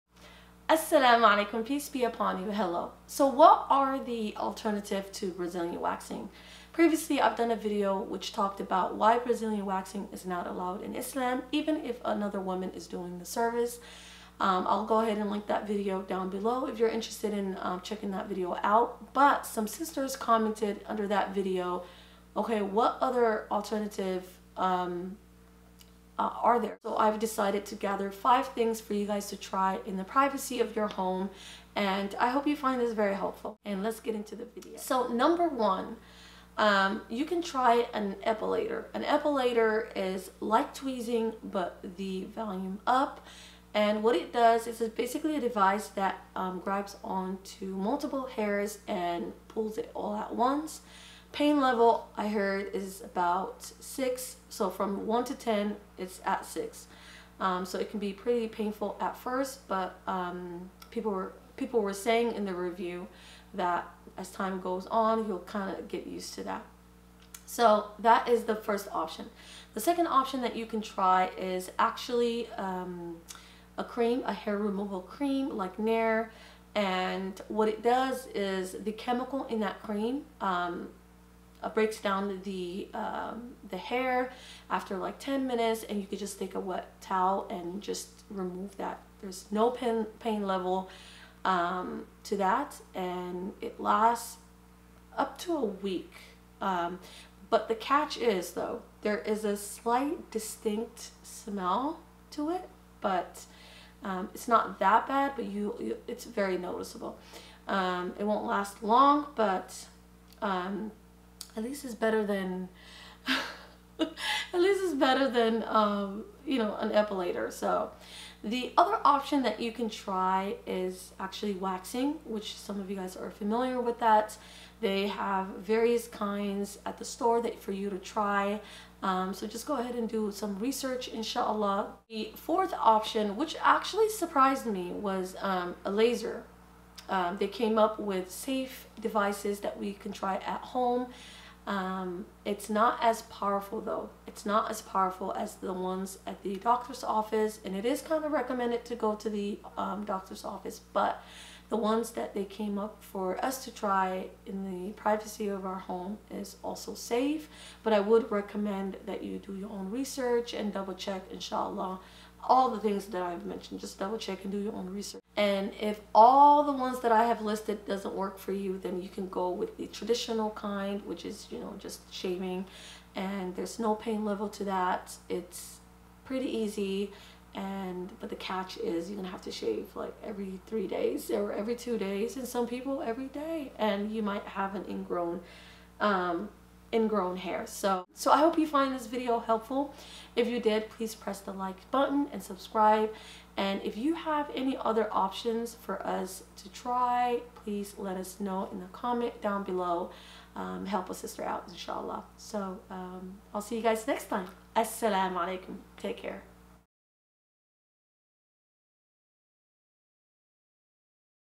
Top 5 better alternatives to Brazilian waxing
If you have been getting Brazilian waxing for awhile and just found out that it is not allowed in Islam, have no worry. I have listed in the video five better alternative for you to try from the privacy of your home. Watch the video to find out!

Previous video:
The truth about Brazilian waxing and the awra among women